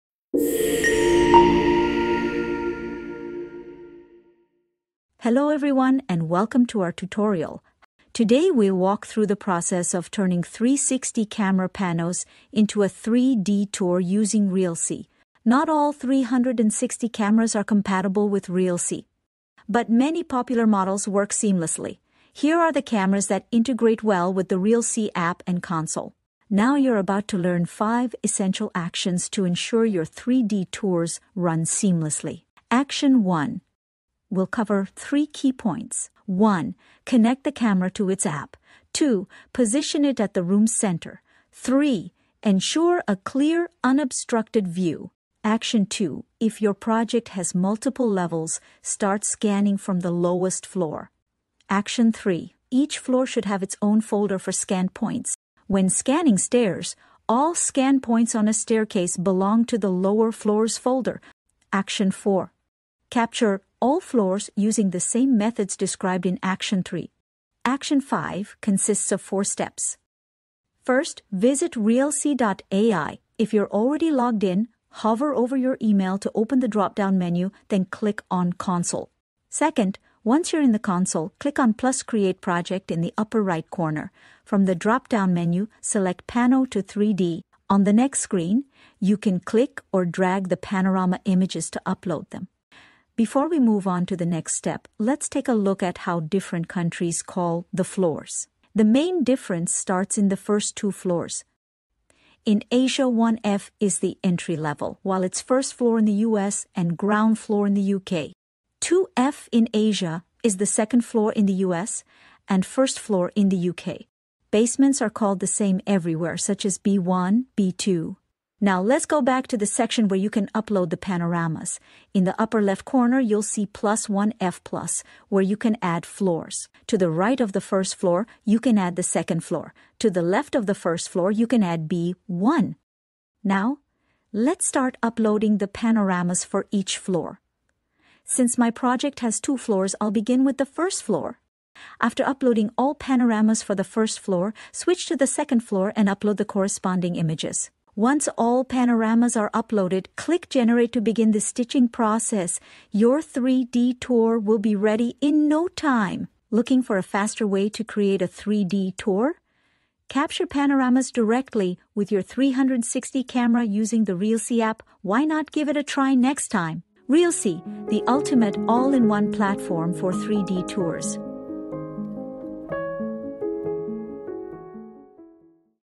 Creating 3D Tours from Panoramic Images : Part 1- Using 360 Cameras
Realsee supports a wide range of 360 cameras, making it easy to transform your panorama images into immersive 3D tours. This video will guide you through a step-by-step process to create stunning 3D experiences.

Realsee – the ultimate all-in-one platform for 3D tours. Watch now and start building your virtual spaces